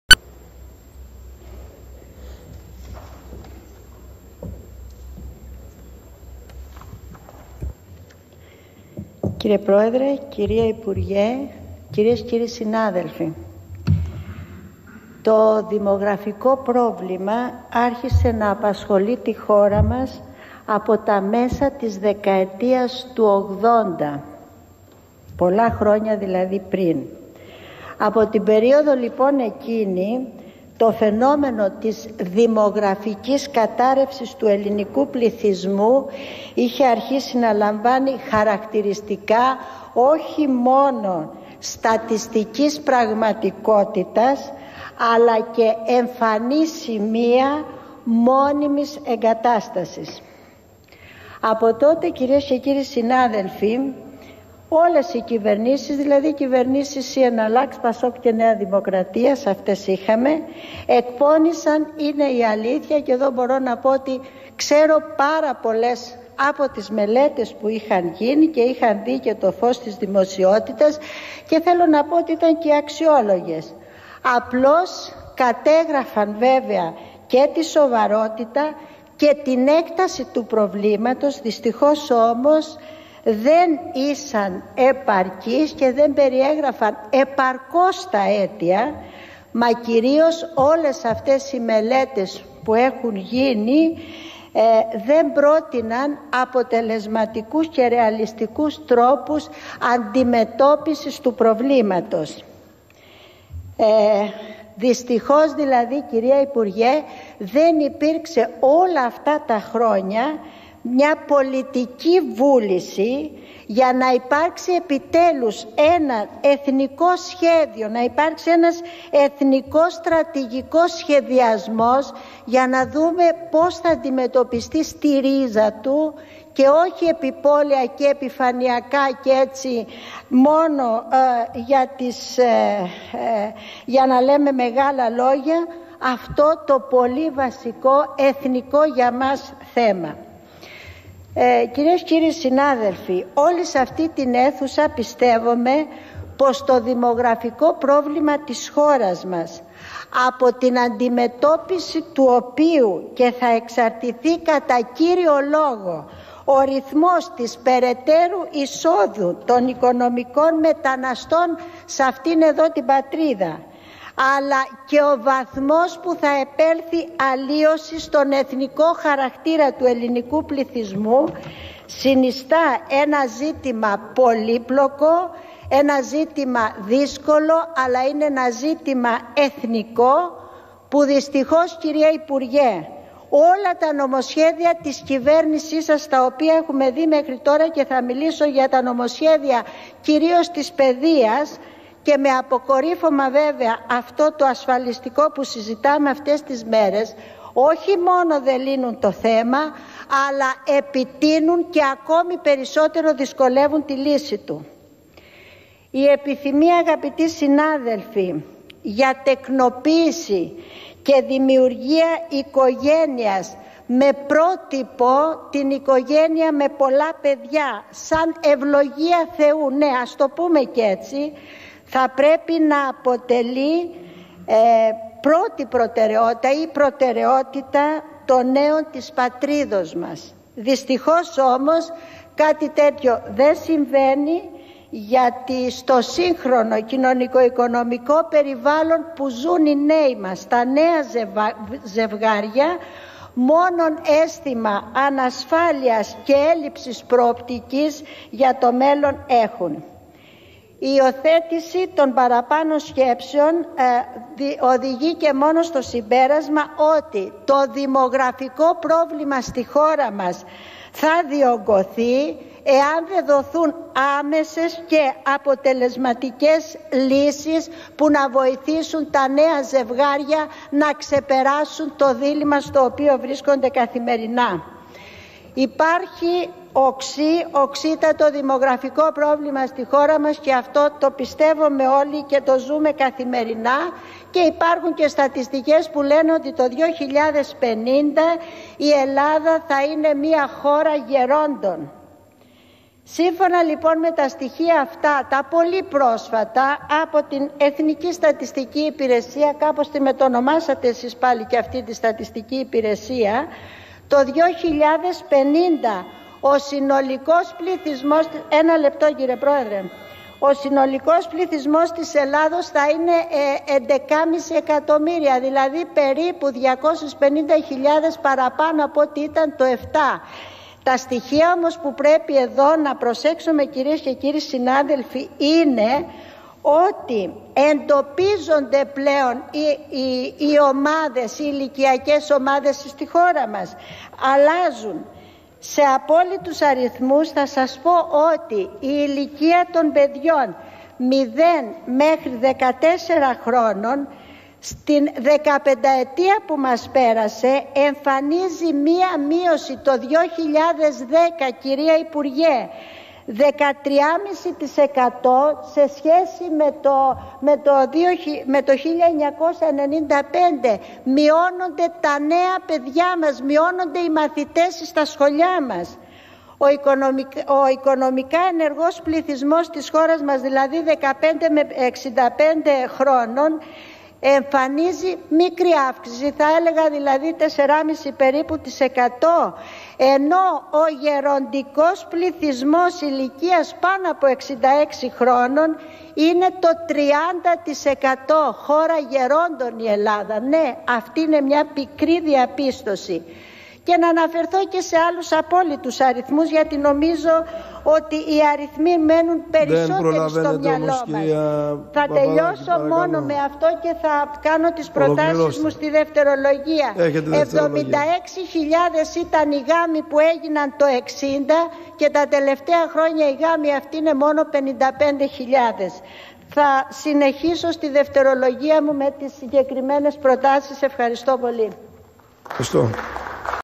ΟΥΡΑΝΙΑ ΠΑΠΑΝΔΡΕΟΥ- ΠΑΠΑΔΑΚΗ - 02 Ιουλ 2010 11:21 π.μ.
ΕΠΕΡΩΤΗΣΗ ΓΙΑ ΔΗΜΟΓΡΑΦΙΚΟ ΠΡΟΒΛΗΜΑ ΤΗΣ ΧΩΡΑΣ